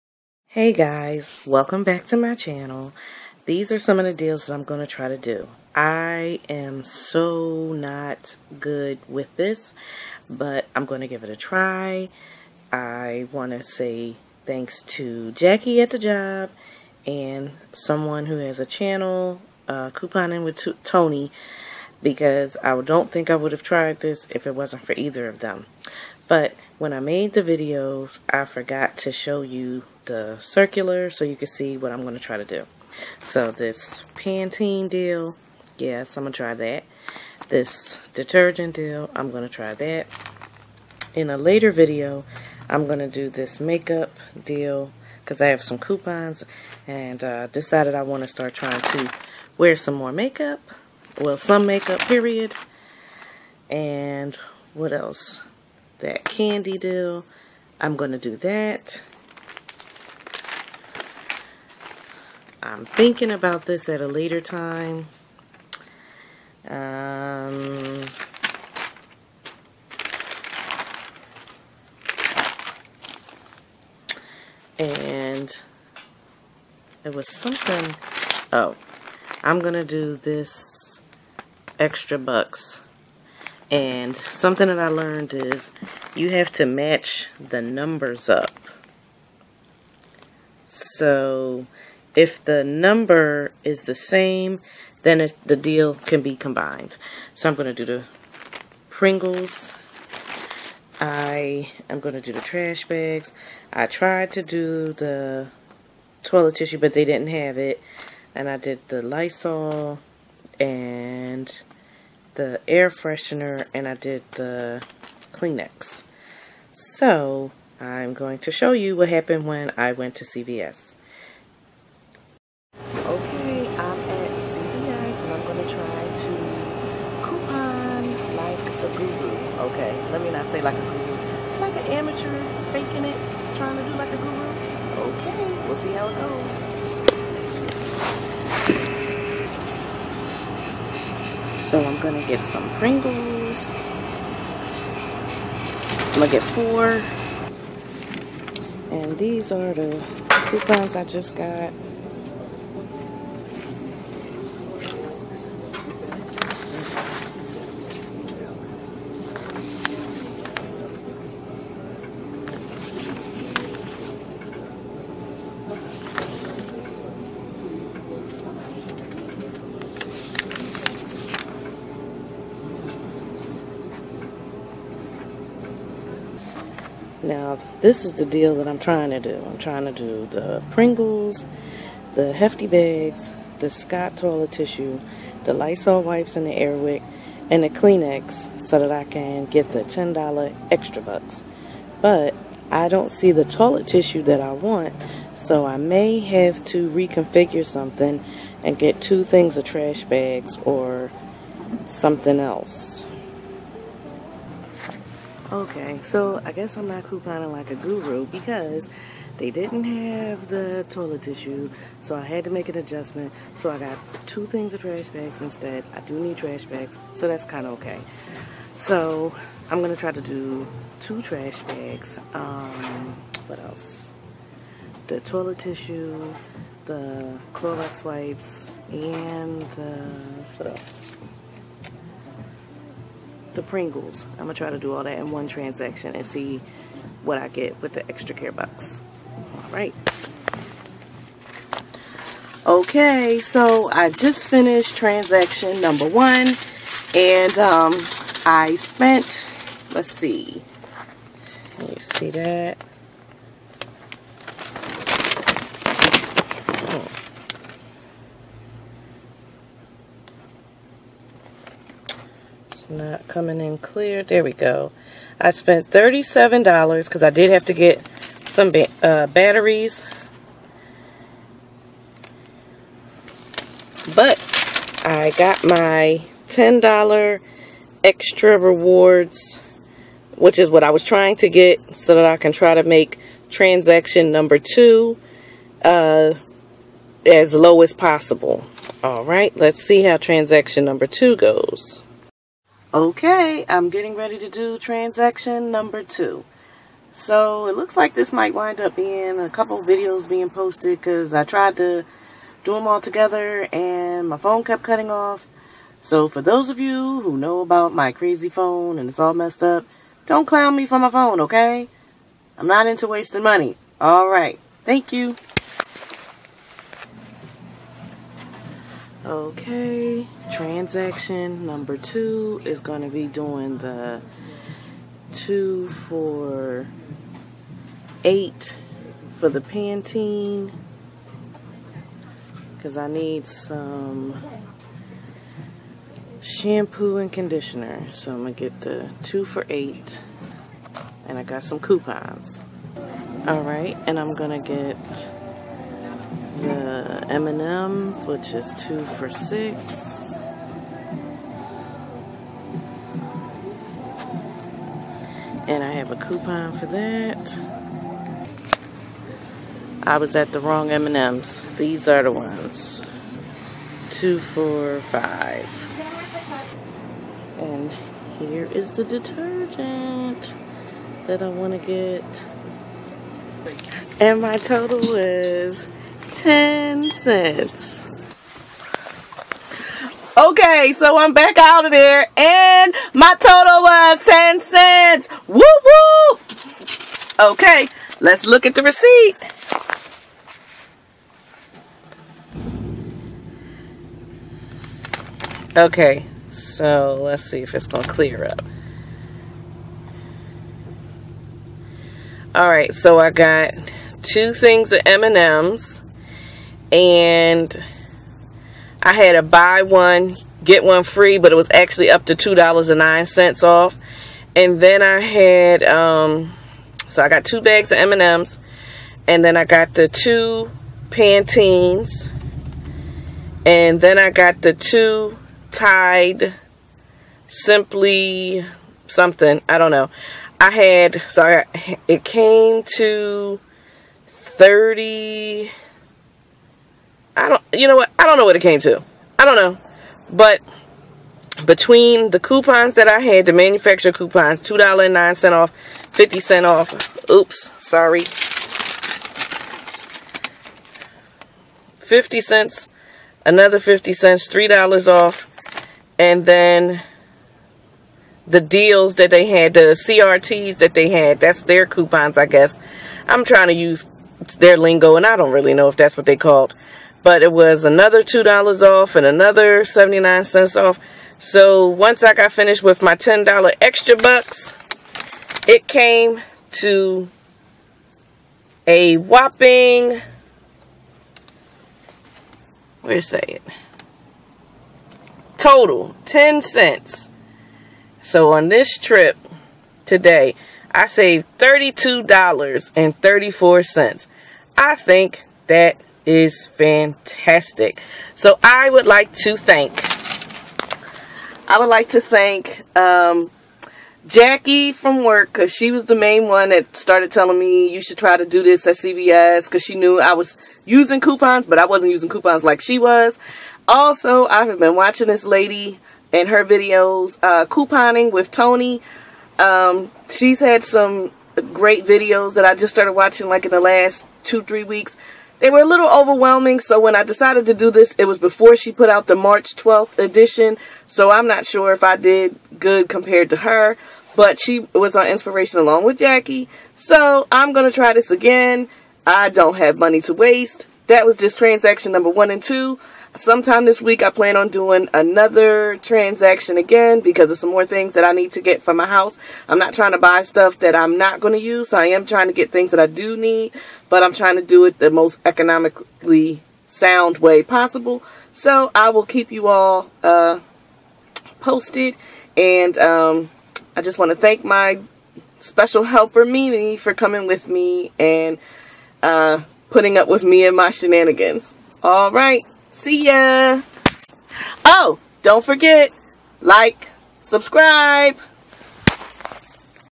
My 1st Major CVS couponing attempt!
In an attempt to smart shop, this is my first foray into CVS couponing.  I am going to try this again.